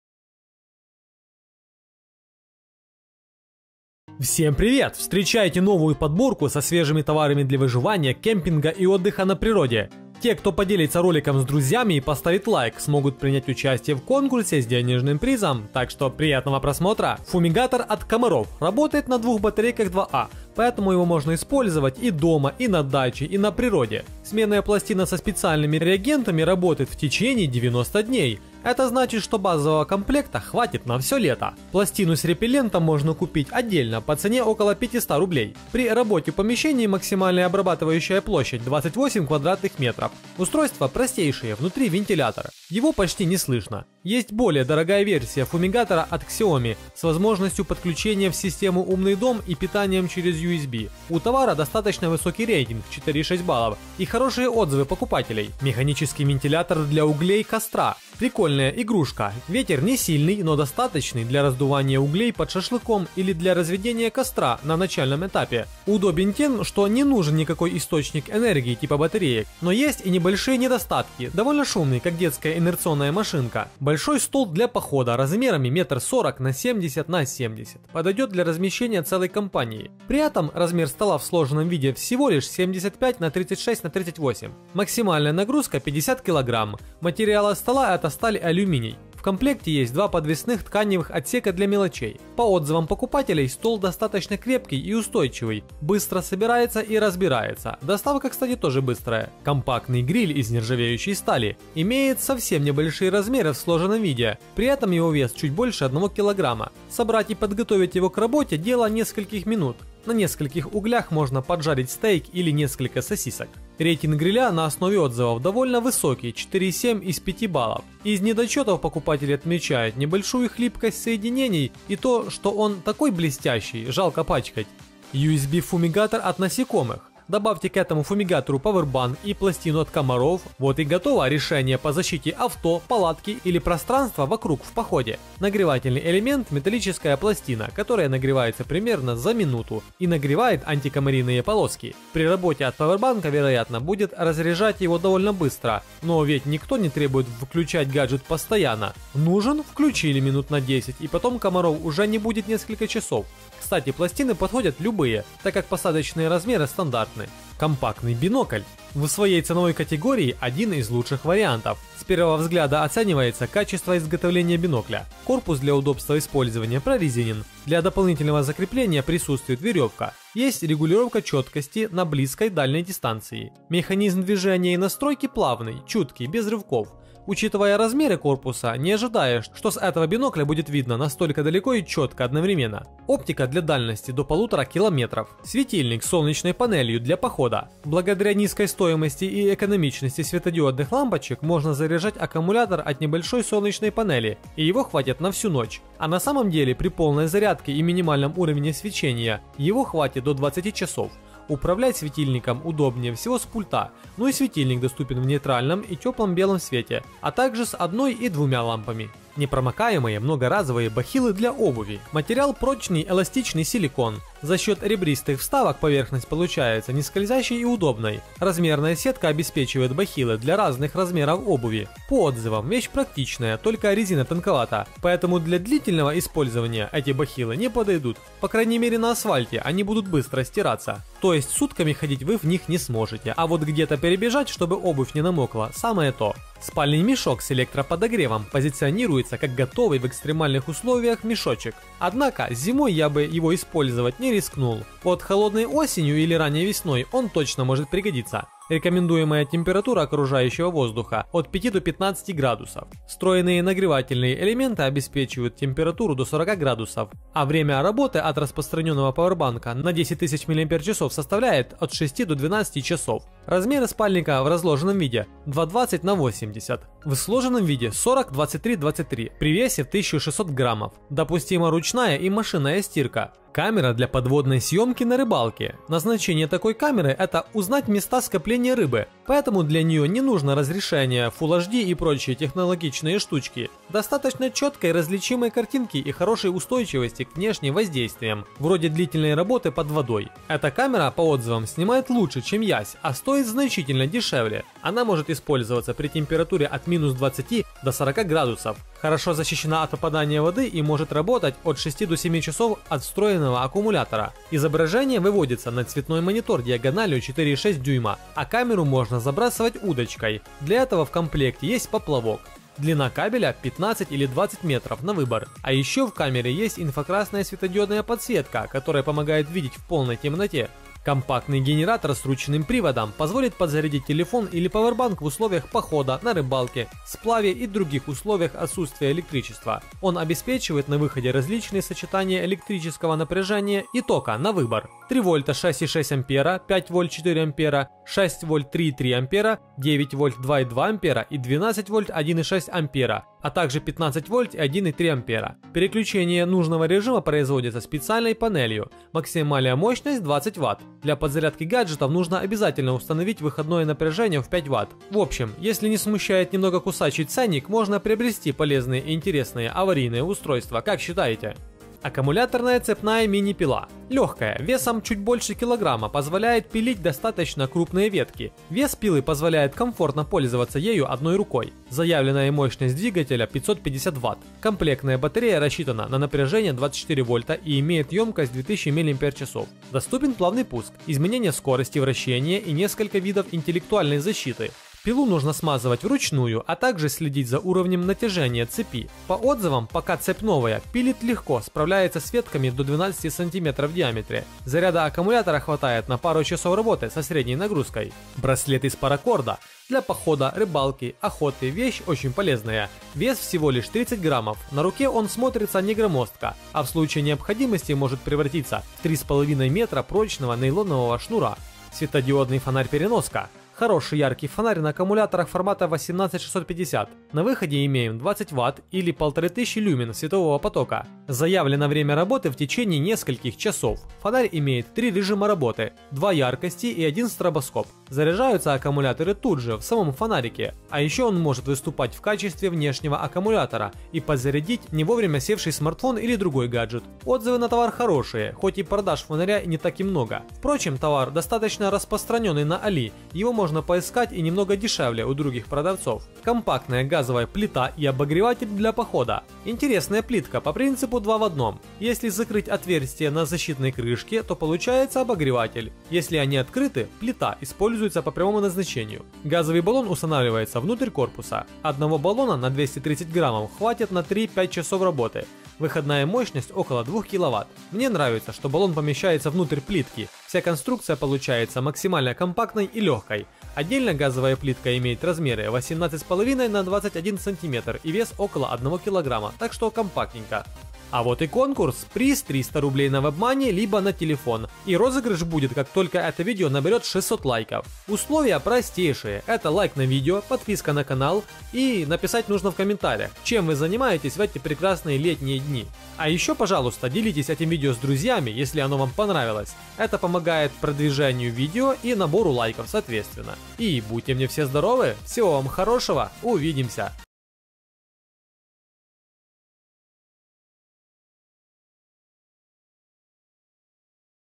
Новые товары Аliexpress - ВСЕ ЧТО НУЖНО В ПОХОДЕ, ПРИ ВЫЖИВАНИИ И НА ПИКНИКЕ + Конкурс
Сейчас я покажу вам 15 таких товаров, которые по моему мнению нужны в походе и не только.

1. ФУМИГАТОР ОТ КОМАРОВ

2. Механический вентилятор для углей и костра

3. Большой складной стол для похода

4. Переносной мини-гриль на угле

5. USB фумигатор от насекомых

7. Светильник с солнечной панелью для похода

8. Непромокаемые многоразовые бахилы для обуви

9. Спальный мешок с электроподогревом

10. Камера для подводной съемки на рыбалке

11. Генератор с ручным приводом

12. Аккумуляторная цепная мини пила

15. Компактная газовая плита и обогреватель для похода

★ КОНКУРС, победителем становится случайный участник.
Для участия нужно:
3. Оставить комментарий под этим видео.
 Приз: 300 рублей на счет мобильного телефона